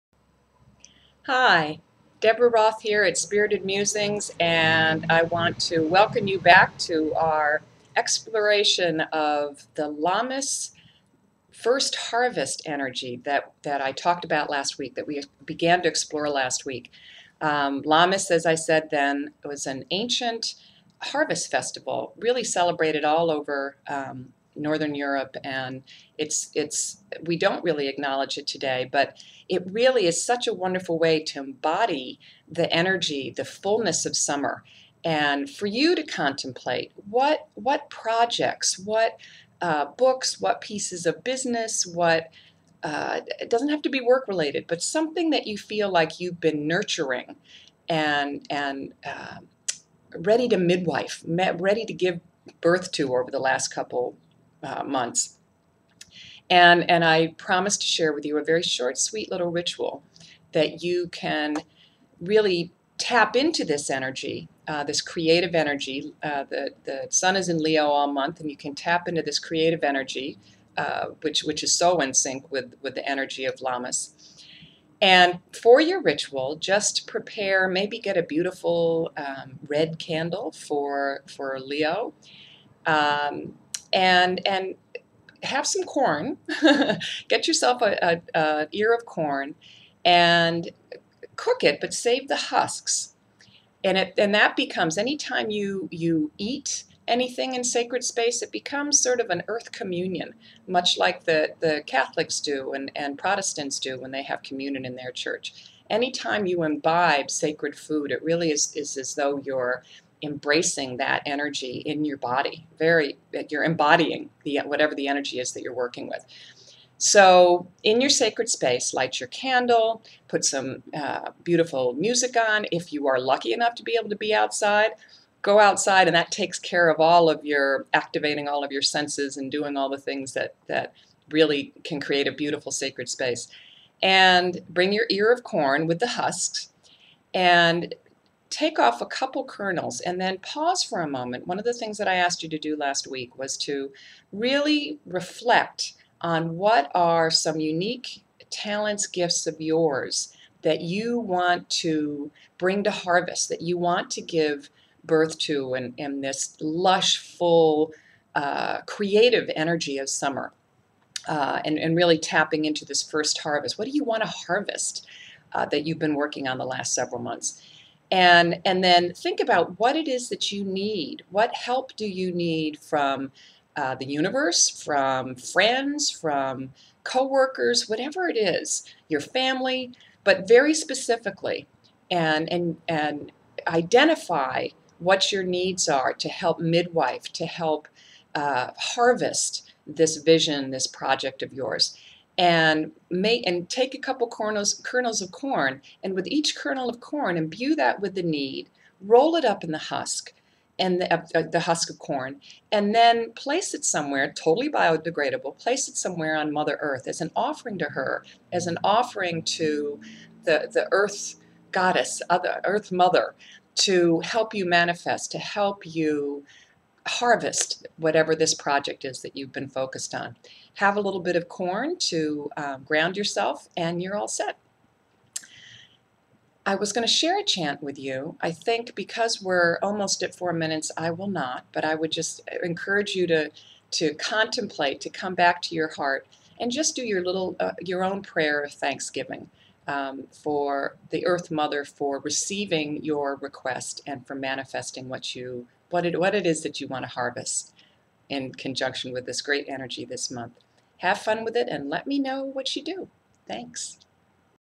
Creating your own Lammas First Harvest ritual
As we celebrate the summer fullness of Mother Earth, contemplate a project, idea, or creation you're ready to "harvest" right now, then get ready to manifest it with a little "corn magic"!
Spirited Reflection: What support do you need in order to "midwife" this intention or vision?